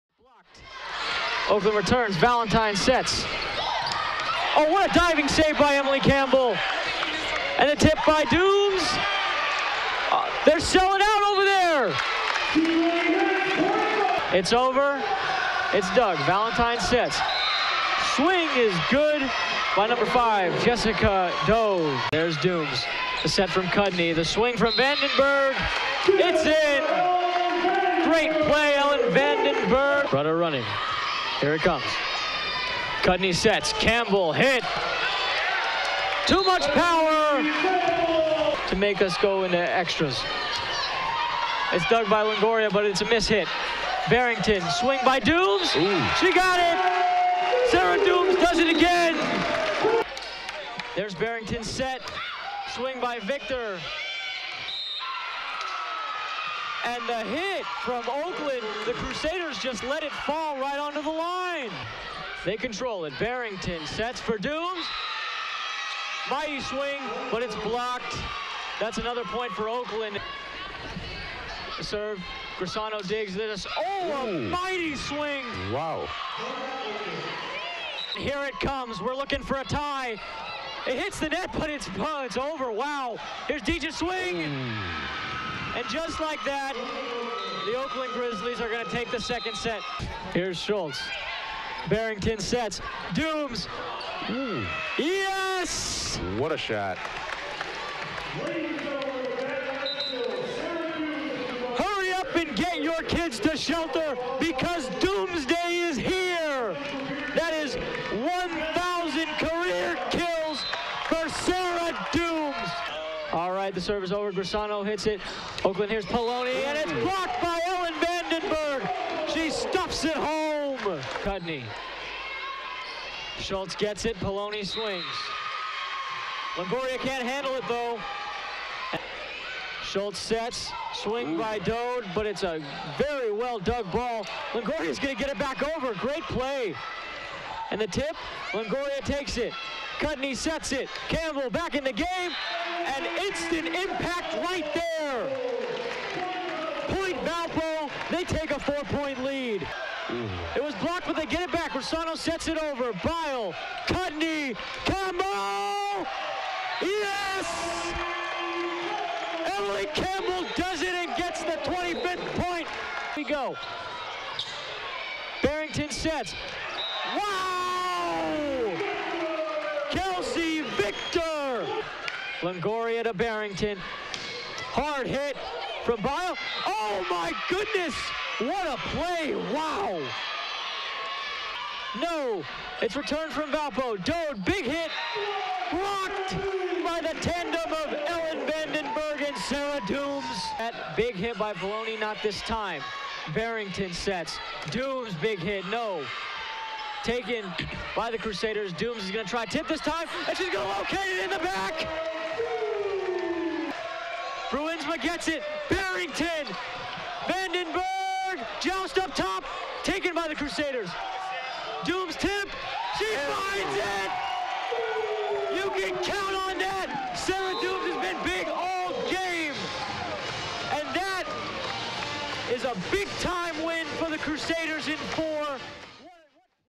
Valpo Crusaders (VB) vs. Oakland Golden Grizzlies
The Valparaiso Crusader Volleyball team hosts Oakland in Horizon League play.